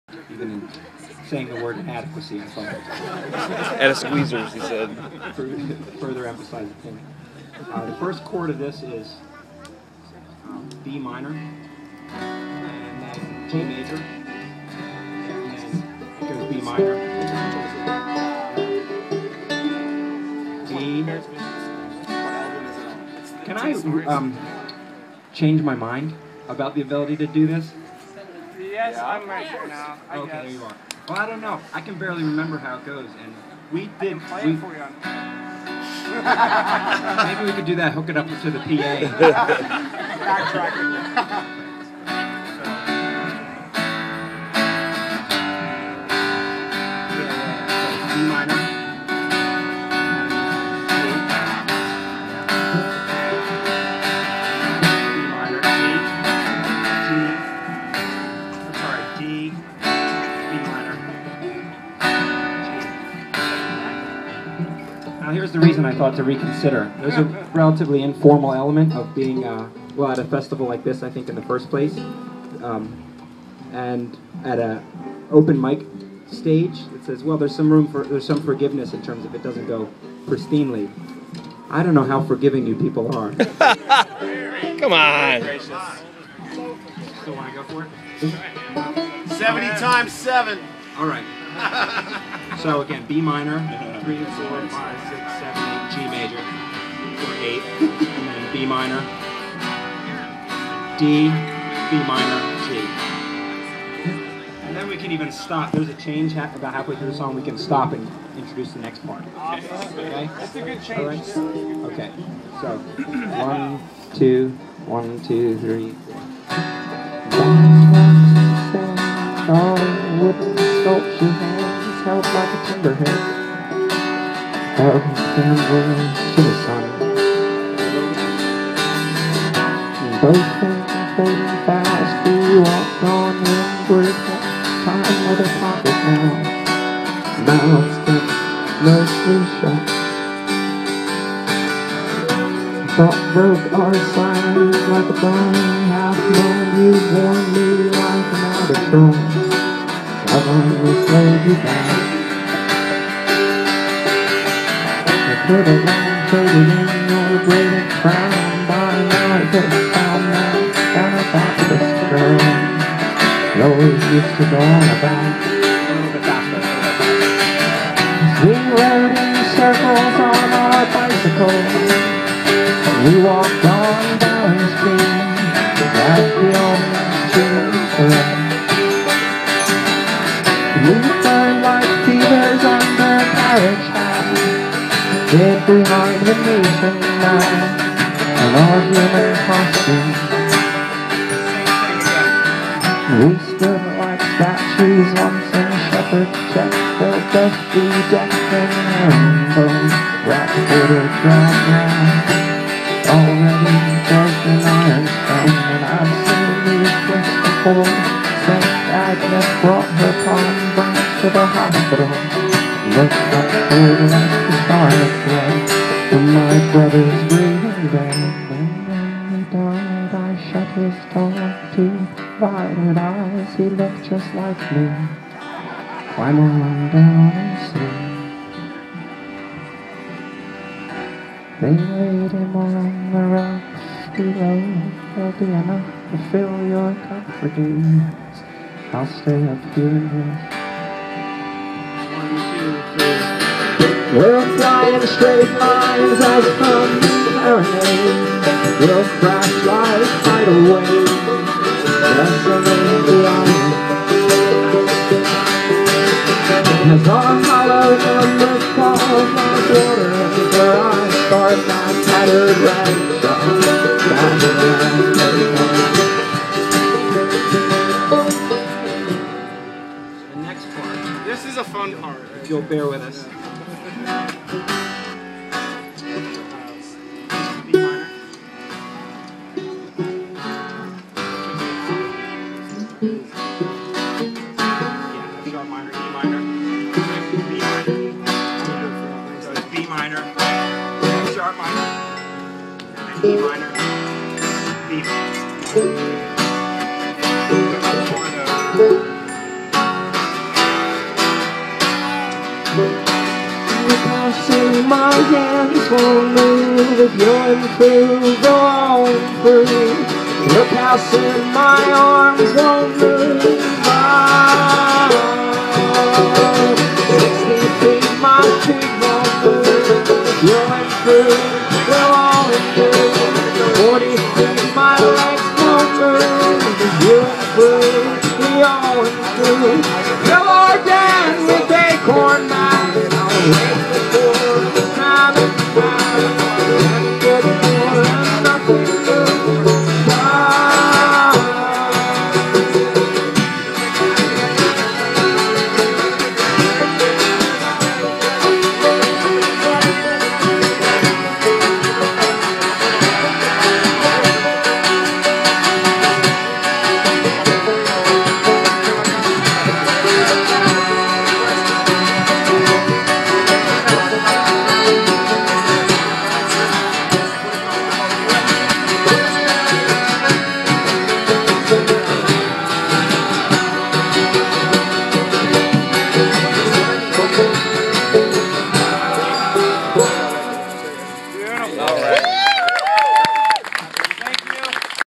aaron weiss from MeWithOutYou and The Collection
Another song from the open mic set at Wild Goose 2012 with Aaron Weiss and The Collection